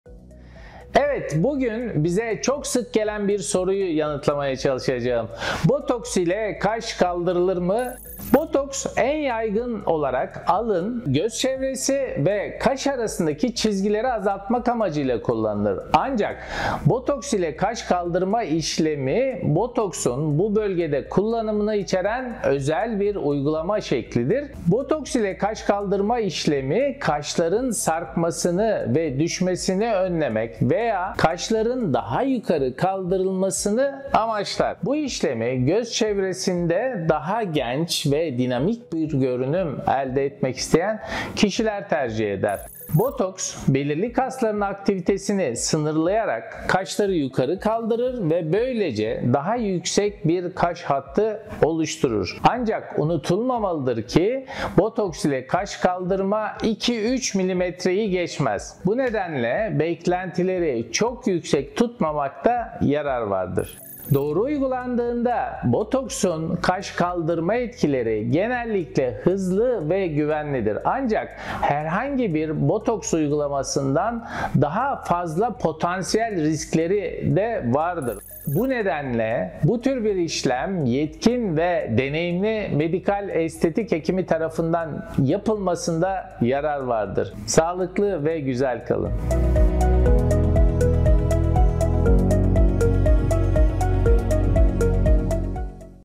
Botoks İle Kaş Kaldırılır Mı? - Dr. Adnan Gürcan
Botoks ile kaş kaldırma işlemi yapılır mı?

Botoks en yaygın olarak alın, göz çevresi ve kaş arasındaki çizgileri azaltmak amacıyla kullanılır. Ancak botoks ile kaş kaldırma işlemi, botoksun bu bölgede kullanımını içeren özel bir uygulama şeklidir.

Botoks ile kaş kaldırma işlemi, kaşların sarkmasını ve düşmesini önlemek veya kaşların daha yukarıya kaldırılmasını amaçlar. Bu işlemi, göz çevresinde daha genç ve dinamik bir görünüm elde etmek isteyen kişiler tercih eder. Botoks, belirli kasların aktivitesini sınırlayarak kaşları yukarı kaldırır ve böylece daha yüksek bir kaş hattı oluşturur. Ancak unutulmamalıdır ki botoks ile kaç kaldırma 2-3mm ‘yi geçmez. Bu nedenle beklentileri çok yüksek tutmamakta yarar vardır.

Doğru uygulandığında, botoksun kaş kaldırma etkileri genellikle hızlı ve güvenlidir. Ancak herhangi bir botoks uygulamasından daha fazla potansiyel riskleri vardır. Bu nedenle, bu tür bir işlem Yetkin ve Deneyimli Medikal Estetik Hekimi tarafından yapılmasında yarar vardır.
Sağlıklı ve güzel kalın.